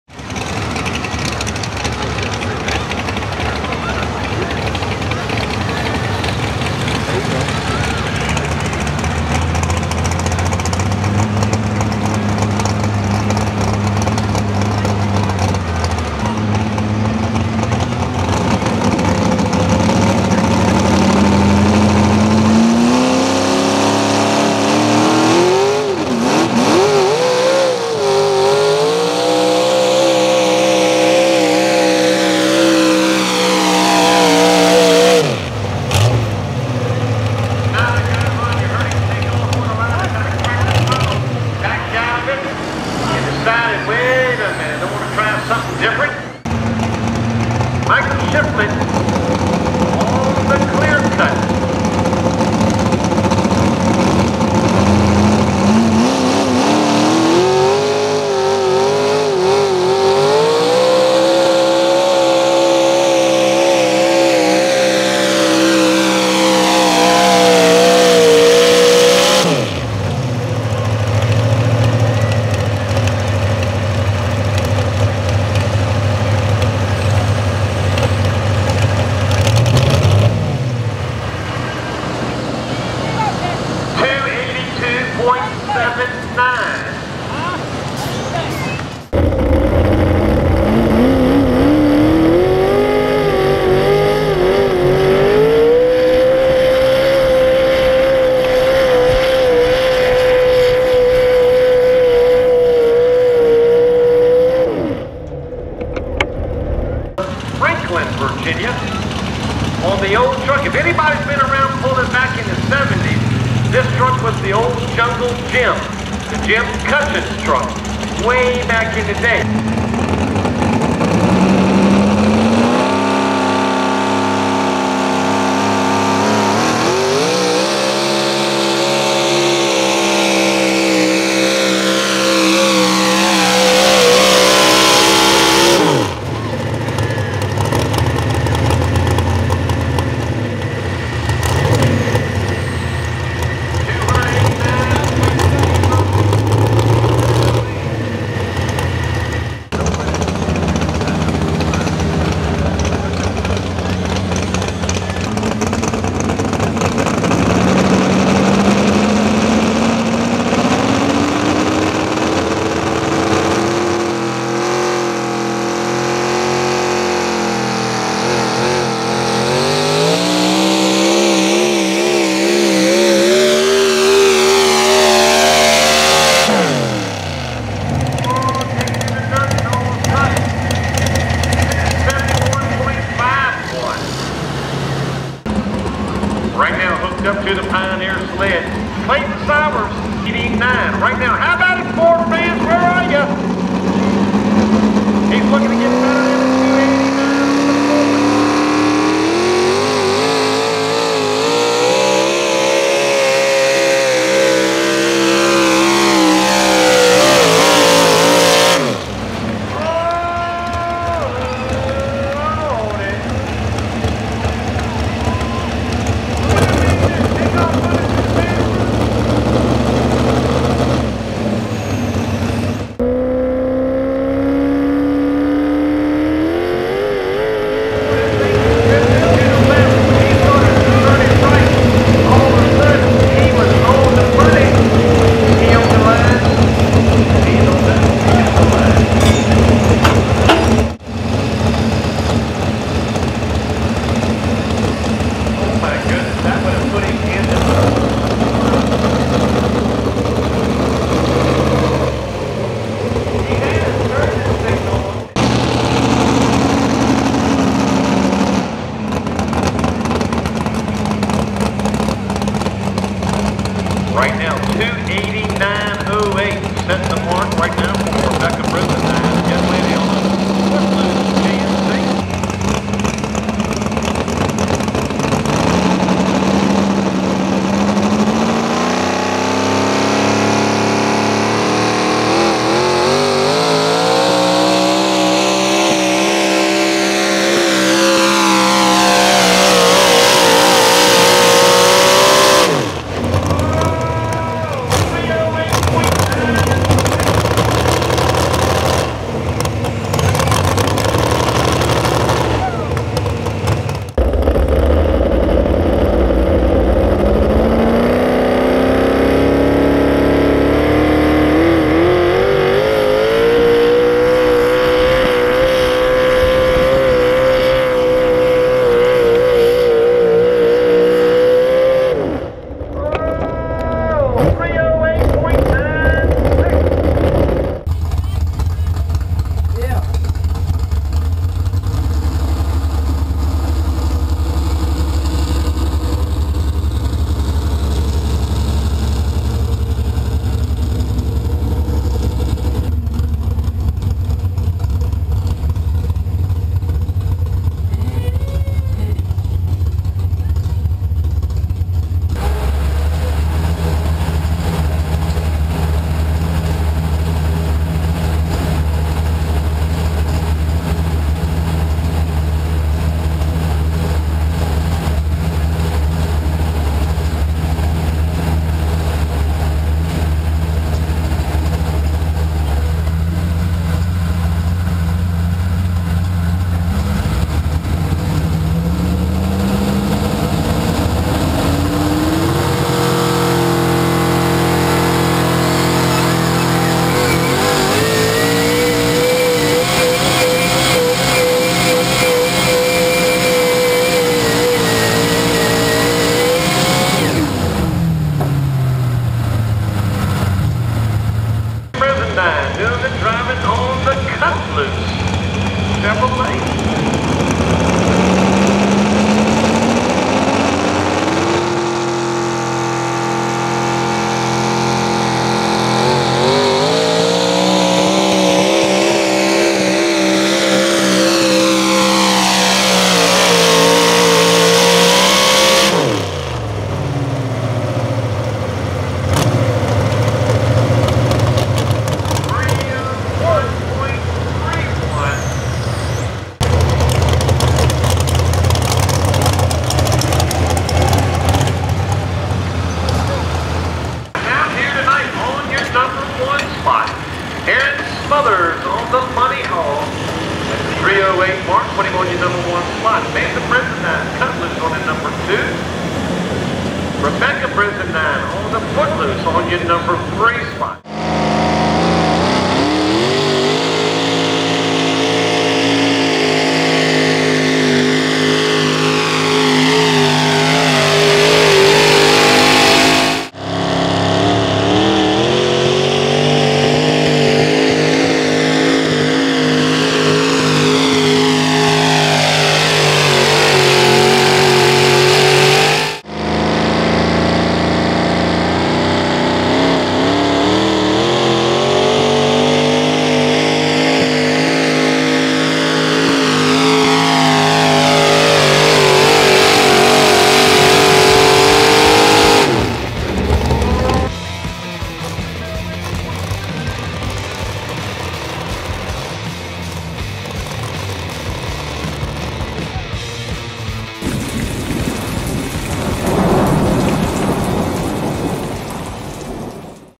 National Sportsman 4x4 at Dillwyn July 27 2019
1. Aaron Smothers "Money Hog"   308.96
2. Mason Brizendine "Cutloose"   301.31
3. Mason Brizendine "Footloose"   289.08
4. Mike Shifflett "Clear Cut"    282.79
5. Phillip Simmons "Foolish Pleasure"  271.51
6. Clayton Sours "Wasted Wages"          DQ/304.58